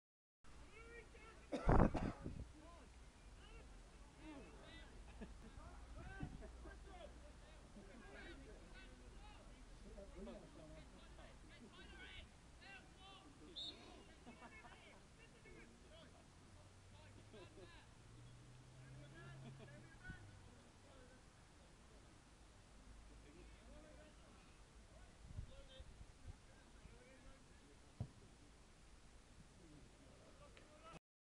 ECPL DIV 1 GAME ,Roche 1 v 0 ST STEPHEN 1st team.
St Stephen 28 Attempts 0 GOALS ,Roche 9 Attempts 1 goal,  Don't you just love Football. 1-0 , Courtney Rowe free-kick is saved by the Roche keeper.